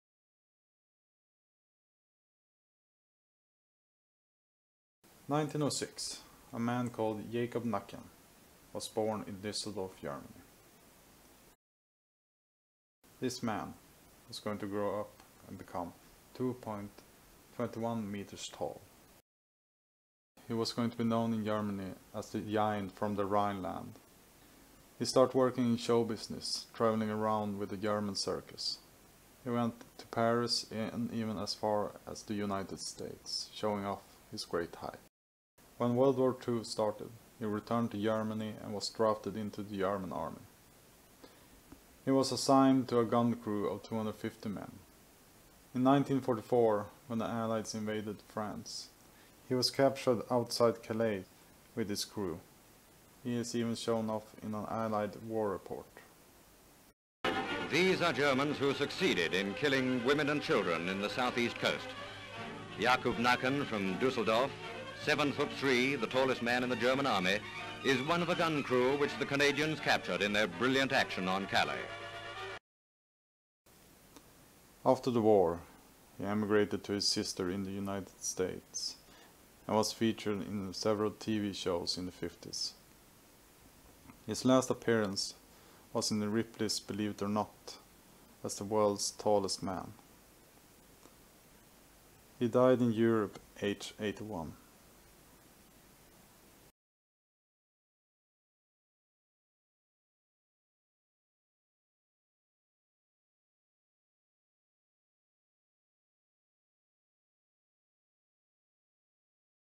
The tallest soldier in the German Army during WW2
Jakob Nacken was the tallest soldier in the German Army during WW2